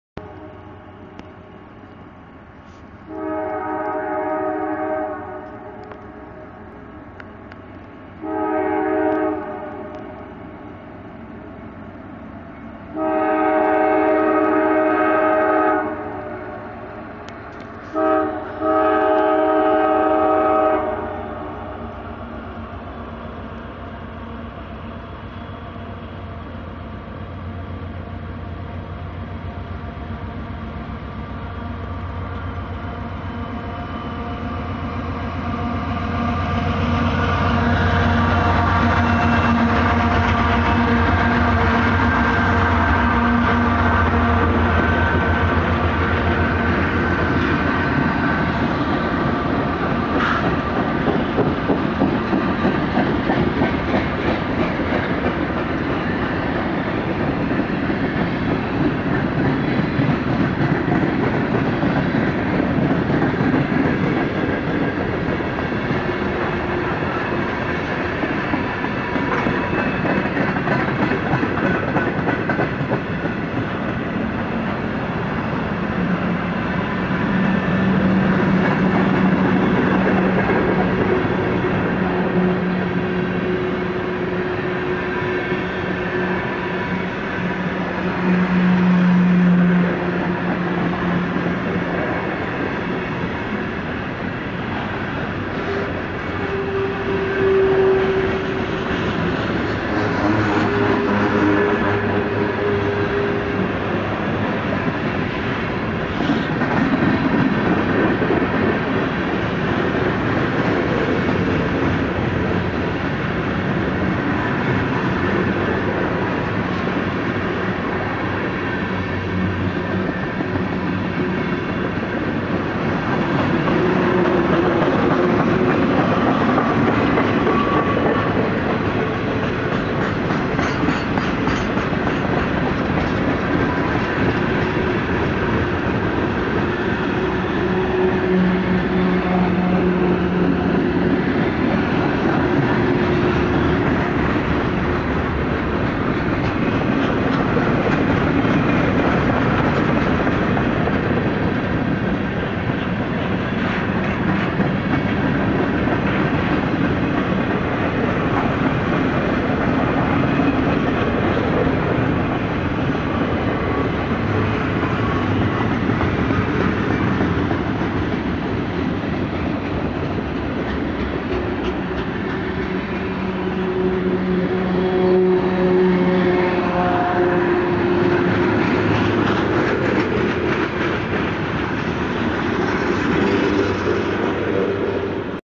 CSX Q693 w/EPIC Ohio Central Power!!! 6-15-11
A pair of CSX AC4400's, Ohio Central GP7, and GP9 lead SB Q693 through Erwin, TN toward Spartanburg, SC. 6-15-11